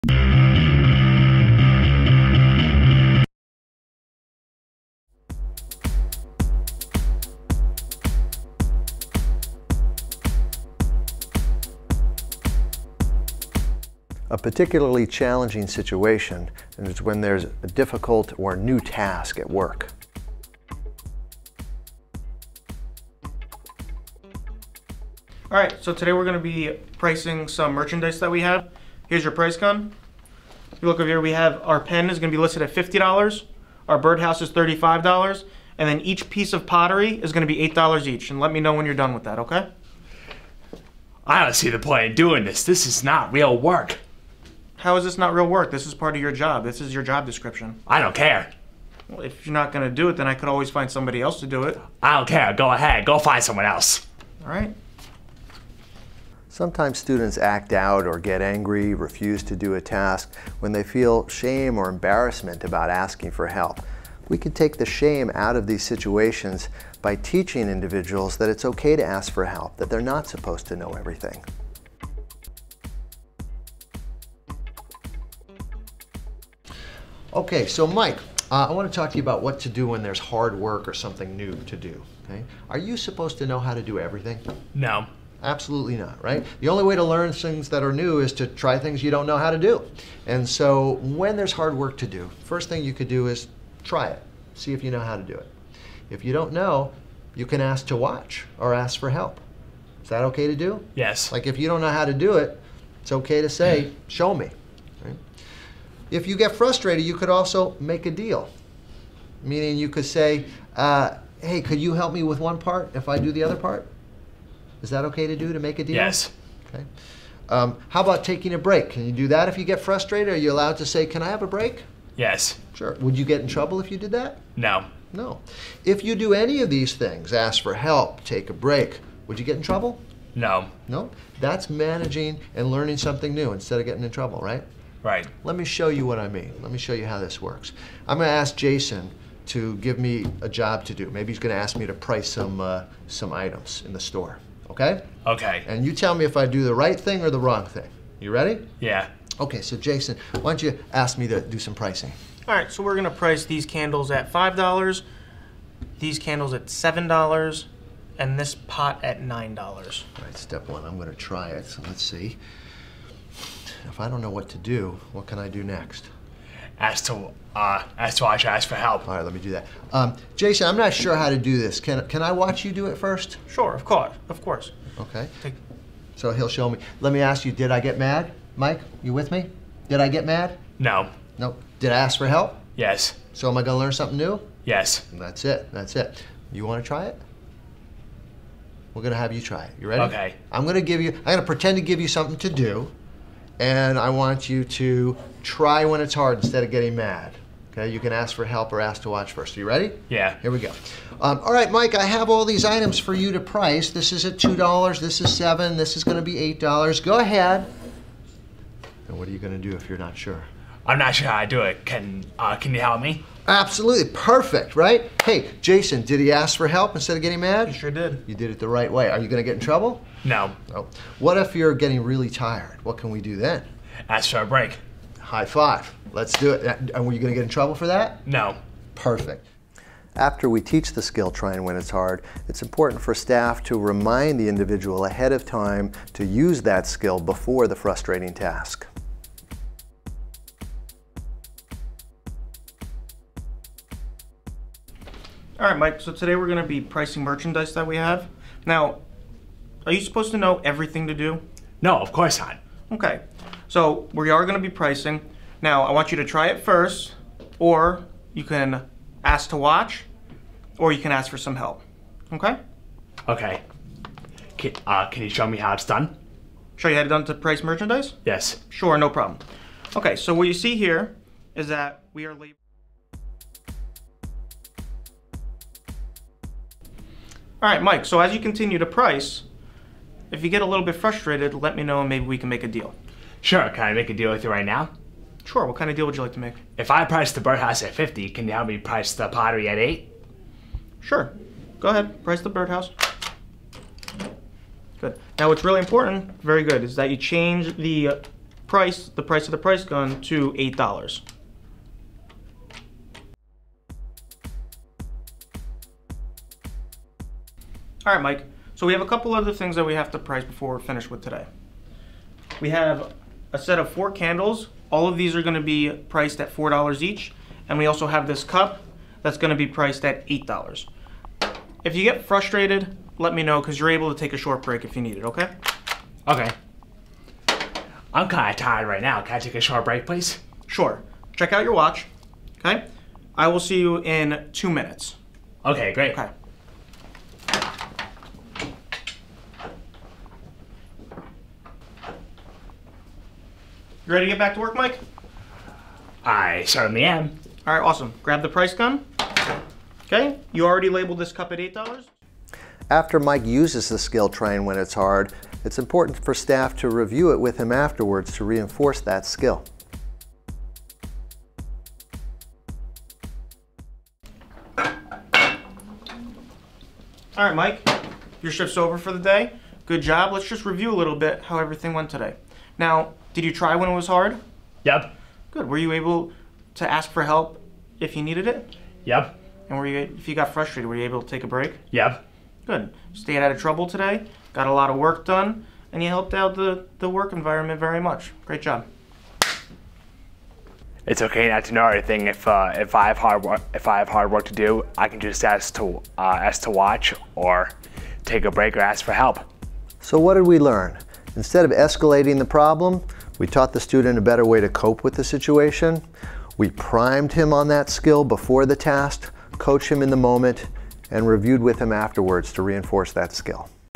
Trying When It's Hard At Work
Demonstrates how to deal with frustrating school work. It explains both what teachers or parents can do to prevent frustration rather then escalate the problem with consequences. It then demonstrates skills the employee can do instead of refusing to do the work. Skills include learning to ask to watch, ask for help, negotiate how much to do, and to request a brief break.